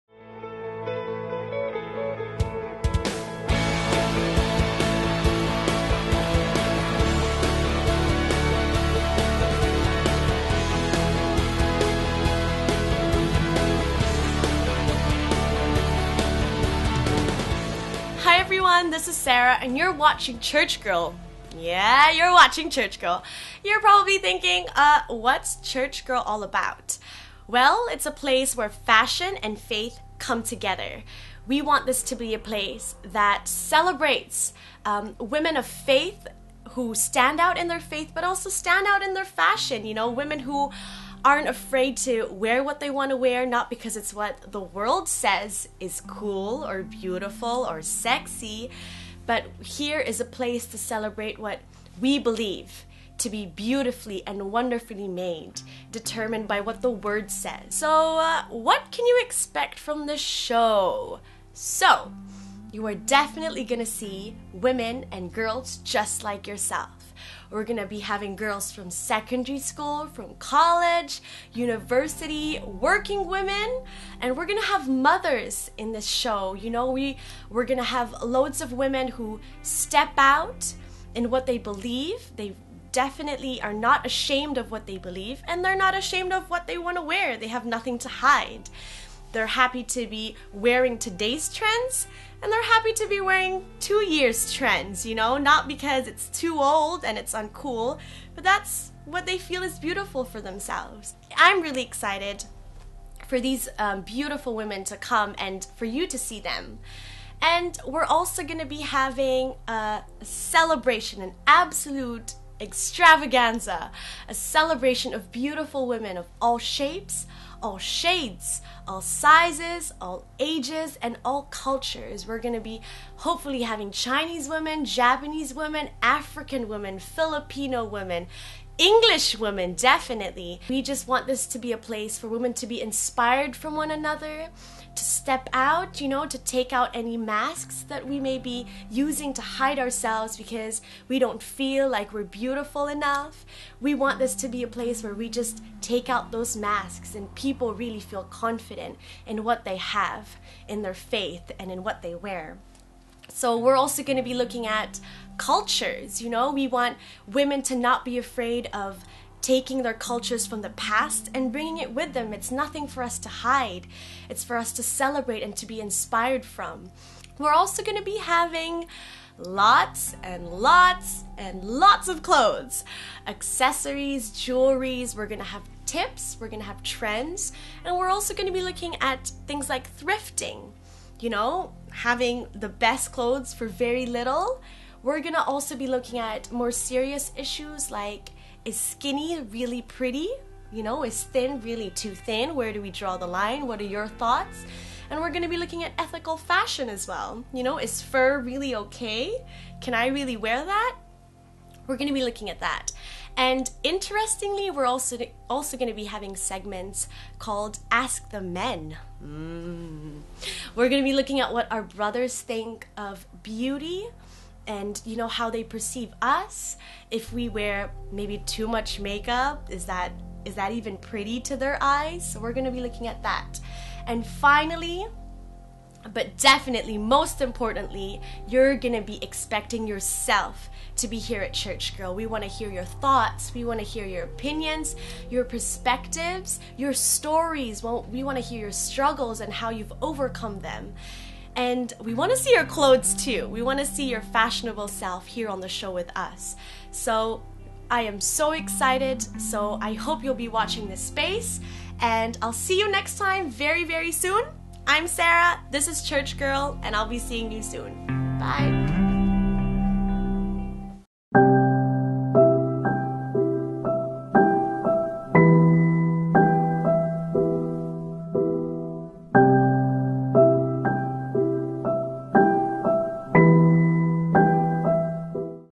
C-GIRL 01 - Watch this space!
Sarah welcomes you to the C-Girl Show and what is coming up in future shows.